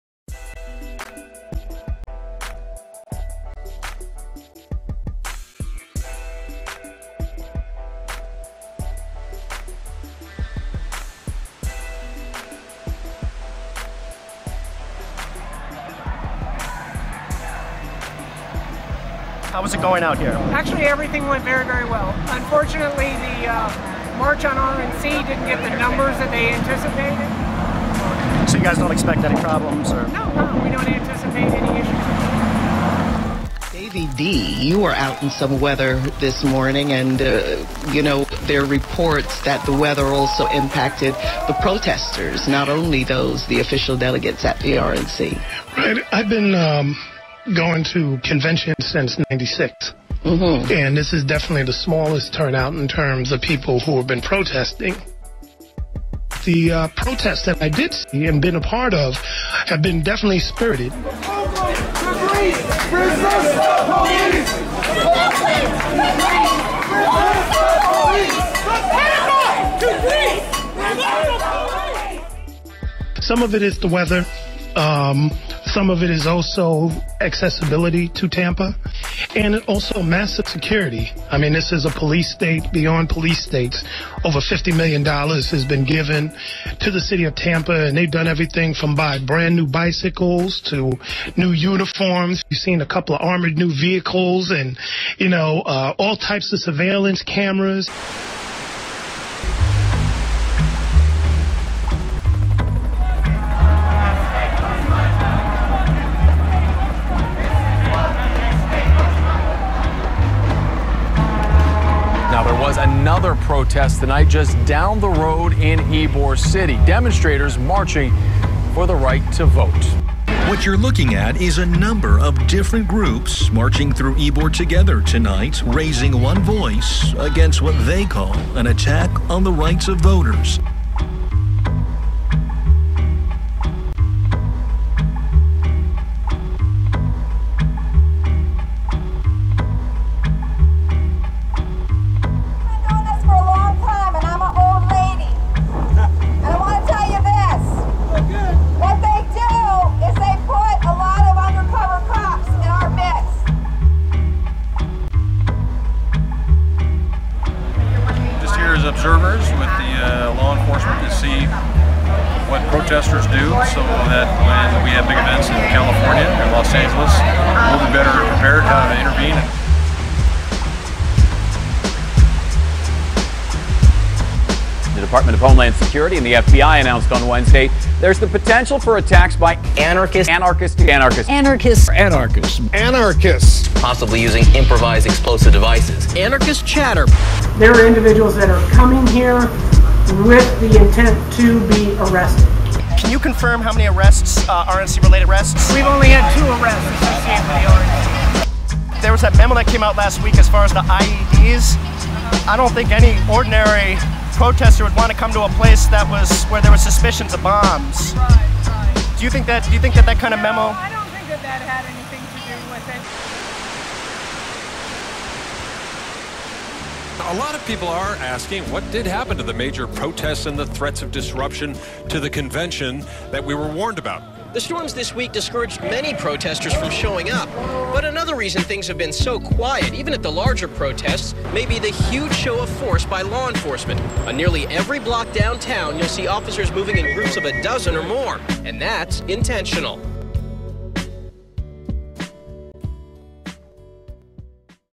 (A28) RNC 2012: Scare Tactics & Protester Perseverance
From Indybay.org
From the media coverage about possible Anarchist Extremists trying to blow up bridges to the overwhelming police presence, the RNC was a major law enforcement exercise bringing together 59 law enforcement agencies to do crowd control together, and at a great cost. This display of force cost 50 million dollars, and as of August 30th has yielded two RNC related arrests citing recently passed laws intended for the RNC.